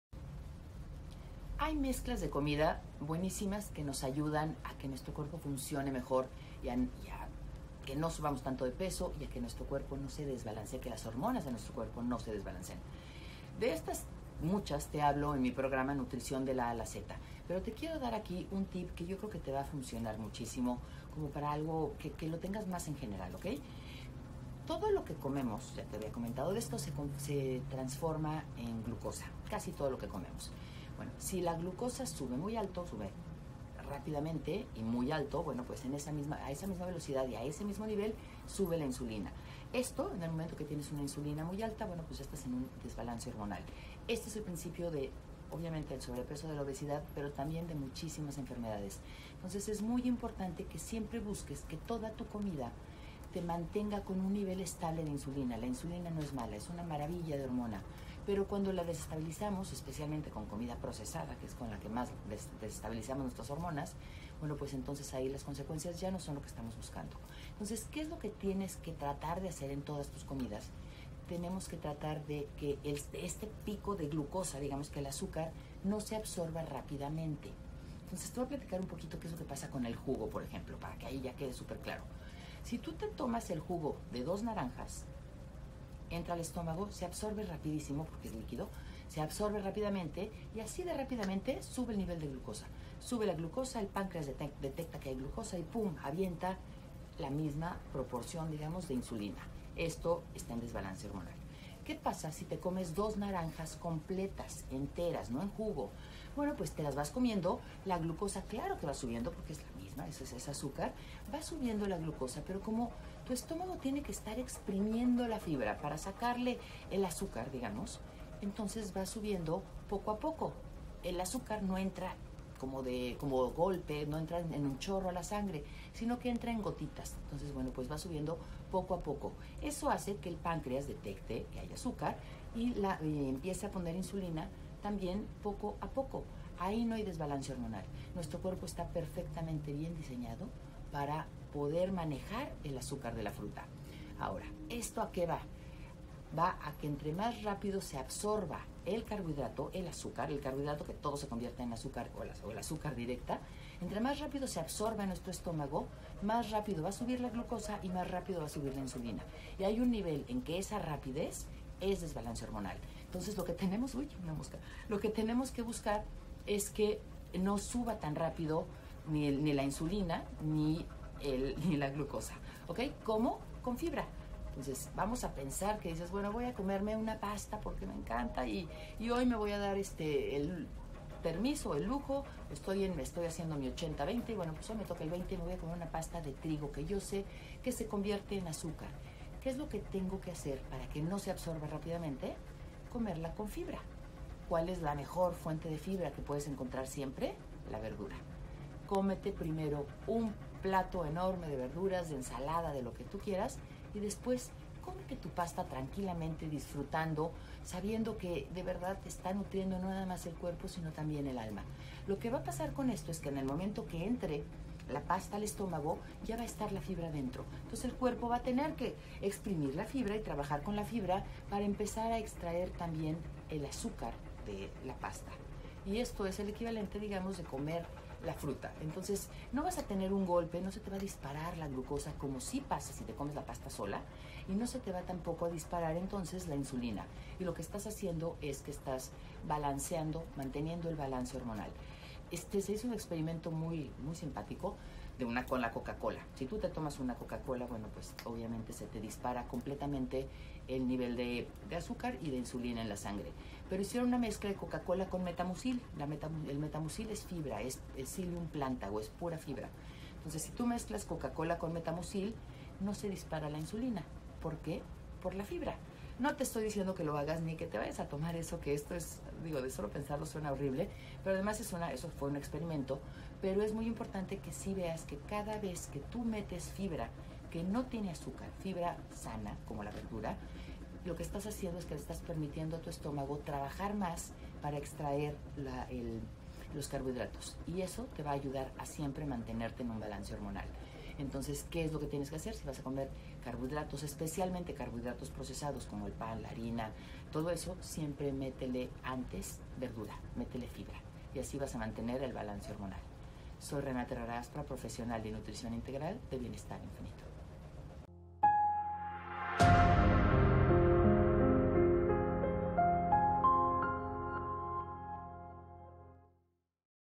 Insulina ¡estable!
Hay ciertas mezclas de comida que nos ayudan a que nuestro cuerpo se mantenga en balance.

Casi todo lo que comemos se convierte en glucosa, cuando sube la glucosa sube la insulina, como ya sabes.

Debemos buscar comida que nos mantenga la insulina estable, o sea que debemos tratar de comer alimentos que no nos suban la glucosa demasiado rápido.

Cuando vamos a comer carbohidratos procesados lo mejor que podemos hacer es comer antes un poco de fibra para mantener estables los niveles de insulina.